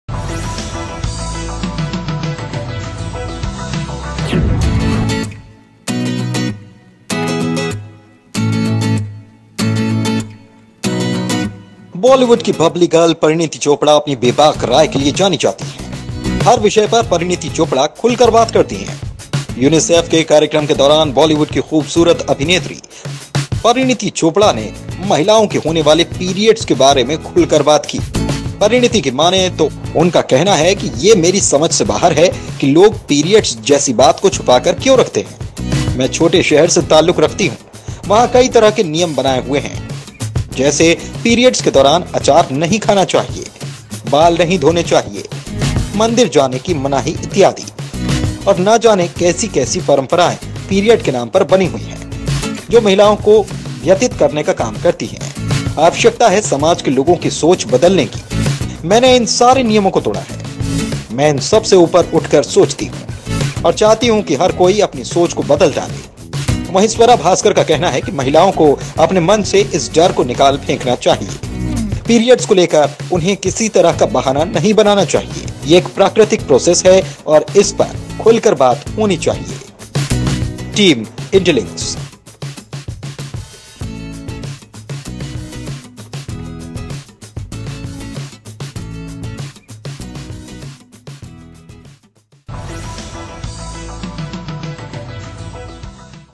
Menstruation। Periods को लेकर Parineeti ने खोले कई राज, बताई चौंकाने वाली सच्चाई
Bollywood Babli Girl Parineeti Chopra is known for her absurd opinion. Parineeti Chopra speaks openly on every subject. During UNICEF's program, Bollywood's beautiful actress Parineeti Chopra spoke openly about women's periods. If Parineeti admits, then he says that 'It is beyond my understanding that why people keep touching things like Periods. I keep in touch with the small city. There are many rules made there. Like during the periods, no conduct should be eaten, hair should not be washed, prohibition of going to temple etc. And do not know what kind of traditions have remained in the name of Period. The women who work for friendship It is necessary to change the thinking of the people of the society. I have broken all these rules. I think above all these above and I think. And want everyone to change their thinking. At the same time, Swara Bhaskar says that women should throw this fear out of their mind. They should not make excuses about periods. This is a natural process. And this should be openly spoken.